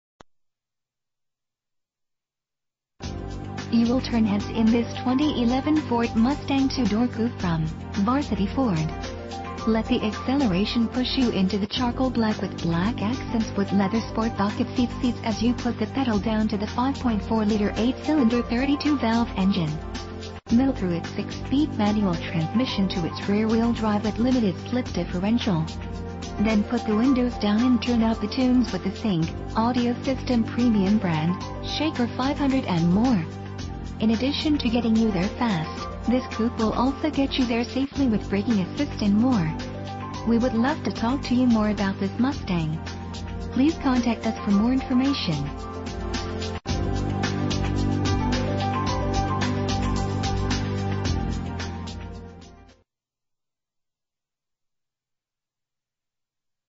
2011 Ford Mustang - Ann Arbor MI
Year: 2011
Make: Ford
Model: Mustang
Engine: 5.4 liter 8 cylinder 32 valve
Transmission: 6-Speed Manual
Exterior: Ebony
Miles: